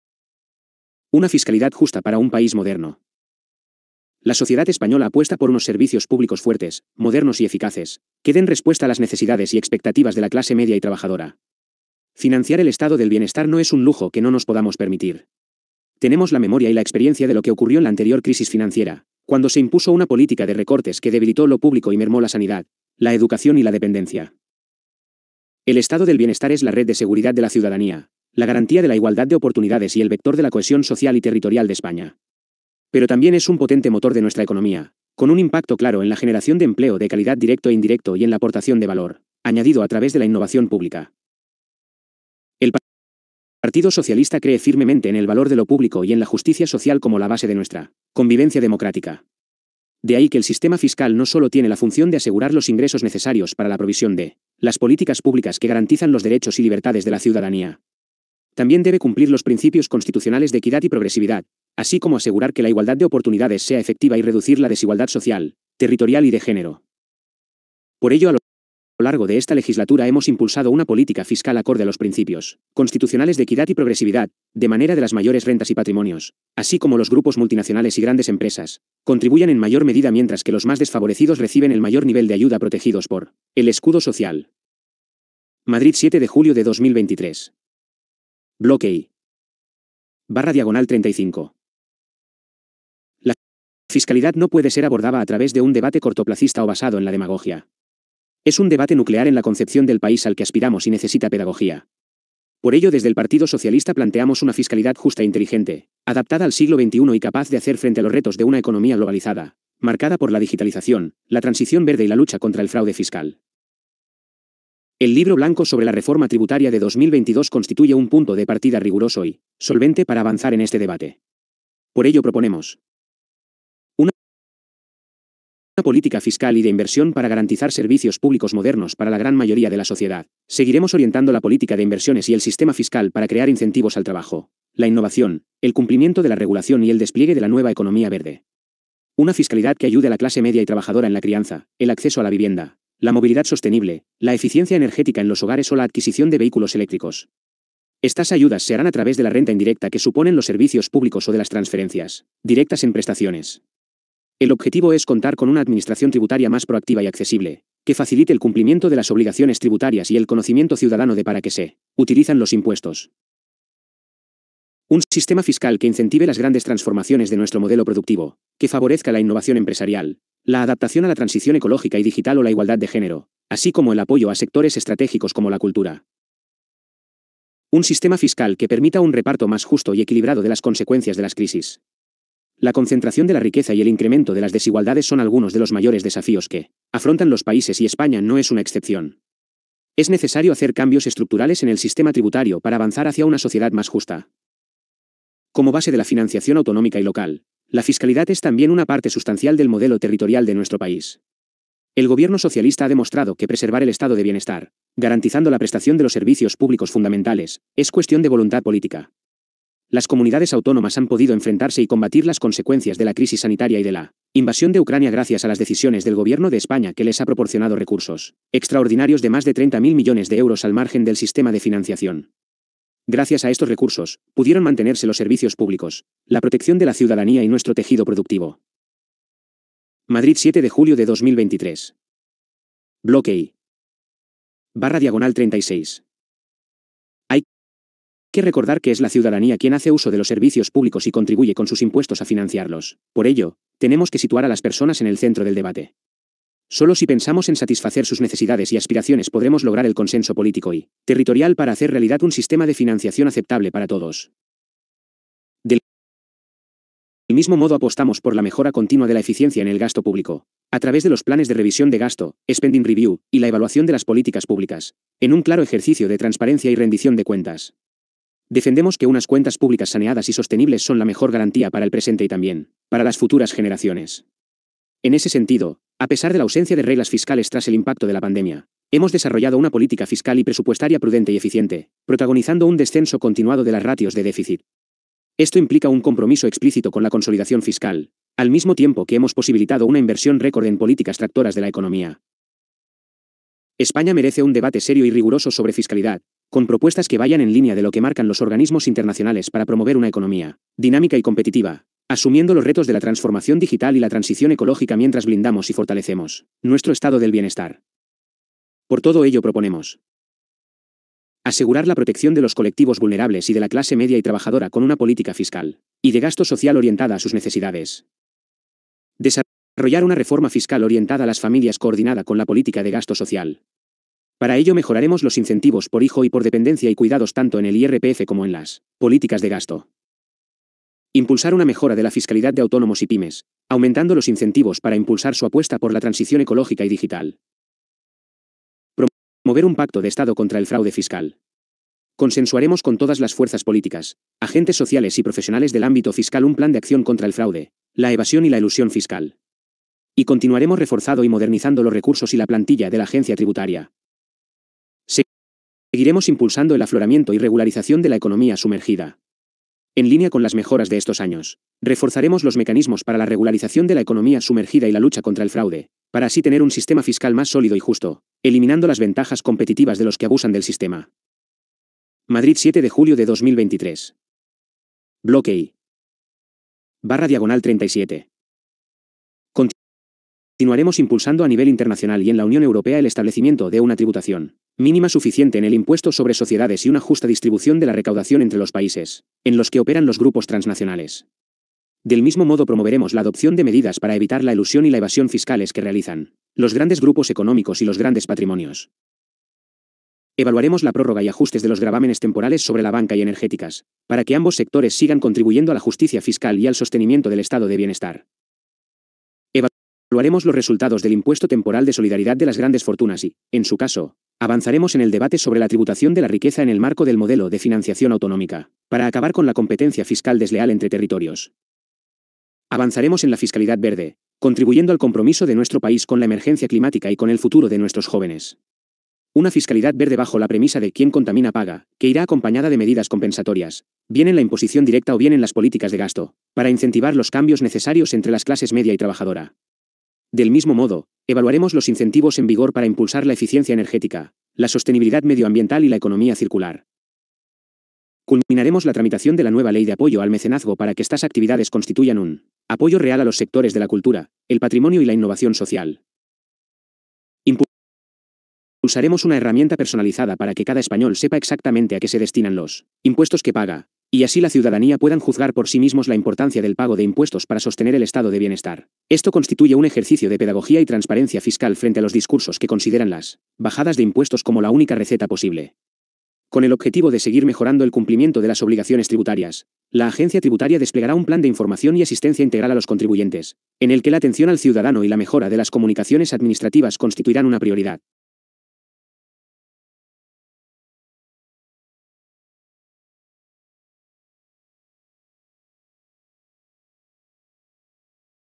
BLOQUE I: FISCALIDAD JUSTA - Lectura del programa de PSOE 2023 - pag. 34 a 37
Lectura del programma de PSOE

Más información:
Página web: Spainbelike.com